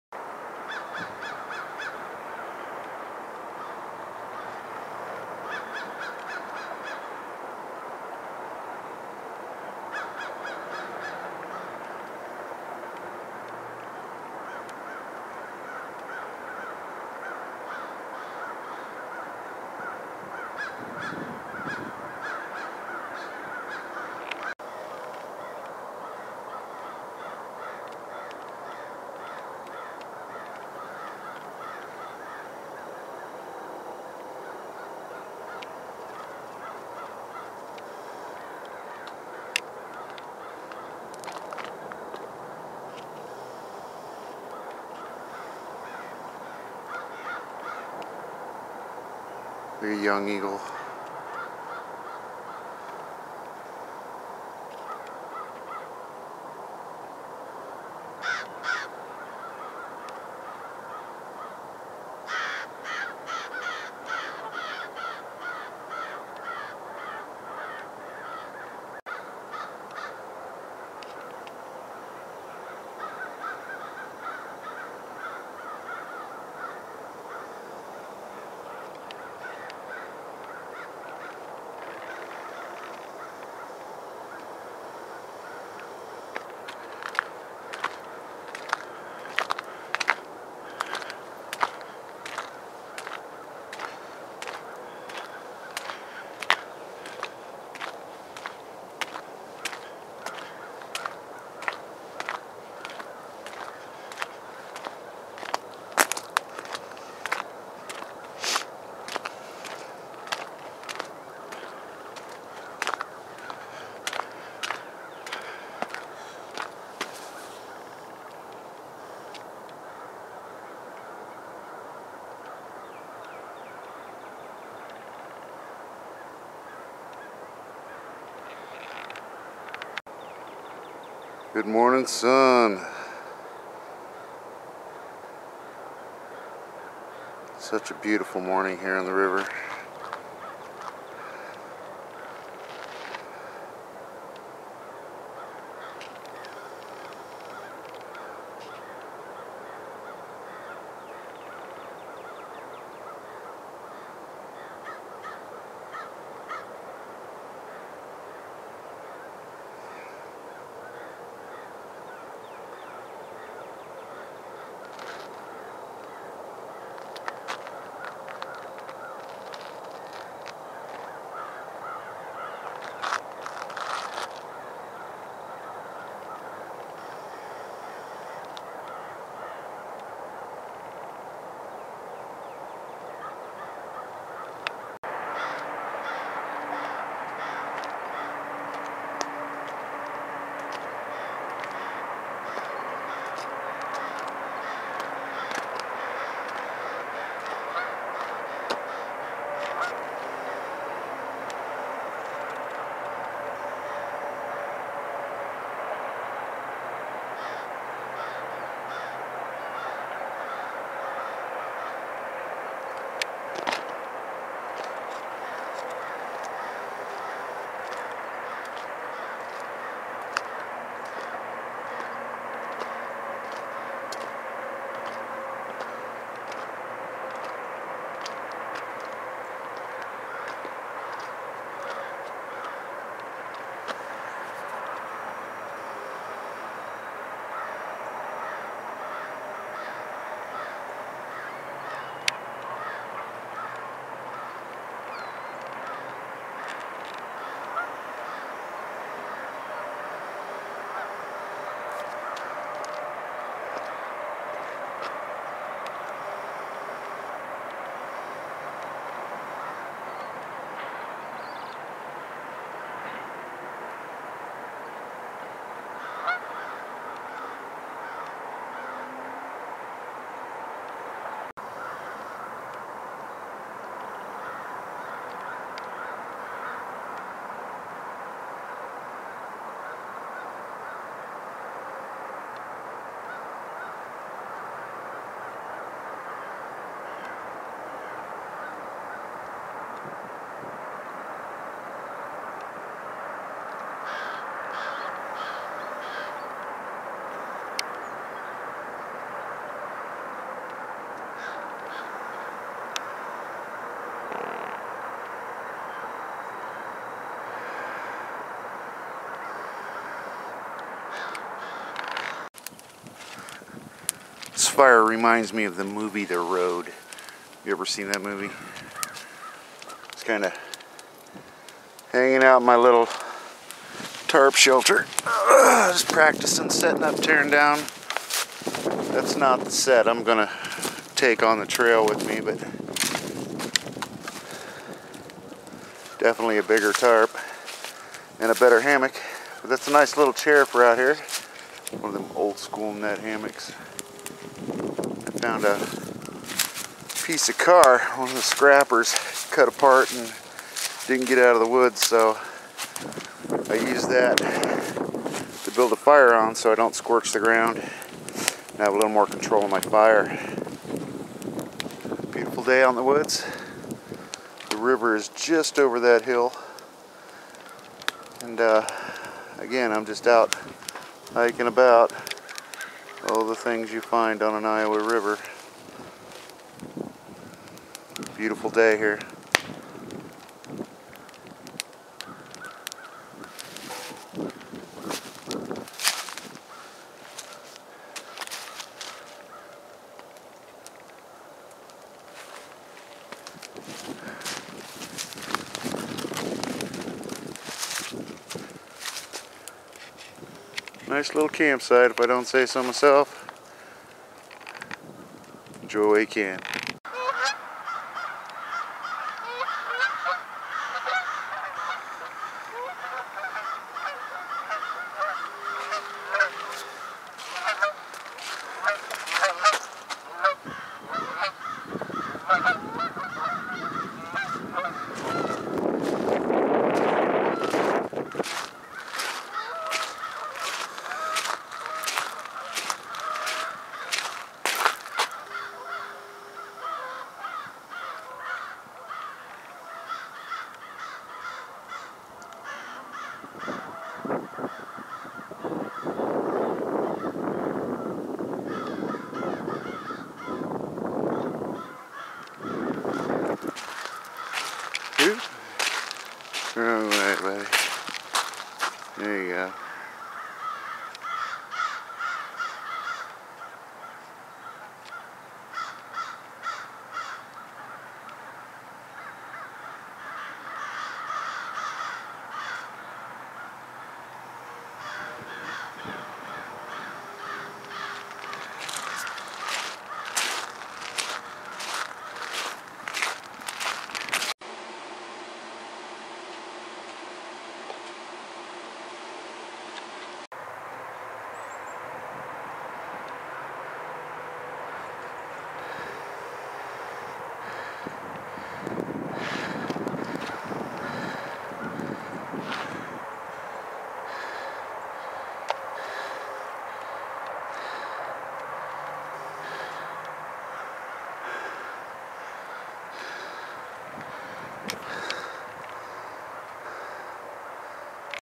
Eagles on the river at Dawn (13 February 2013) Hiking on Des Moines river
What a beautiful day to wake up before dawn and hike a few miles to video eagles having breakfast. A little camp set up practice, for my Appalachian Trail section hike, and a fire worthy of "The Road". lol
I can honestly say I prefer my mud frozen and not thawed.. Mud Makes the workout that much more fun. No pain no gain... enjoy